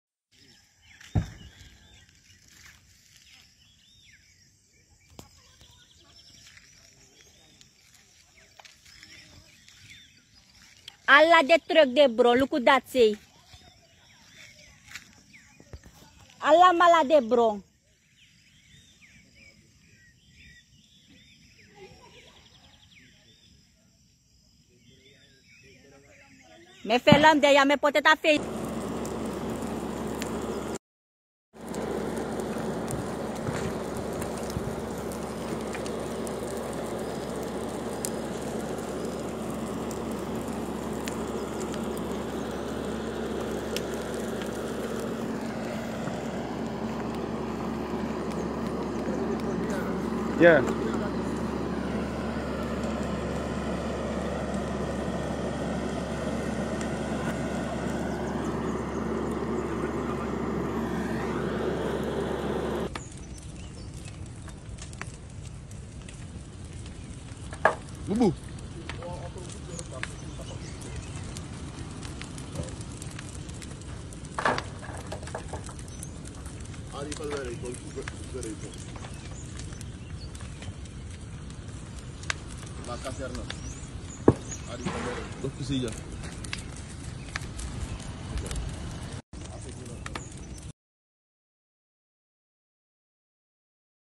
Brandstichting en schoten gelost te Pikin Saron
Chaotische situatie Pikin Saron: Houttrucks en auto's in brand gestoken, schoten gelost, politieagenten gewond geraakt.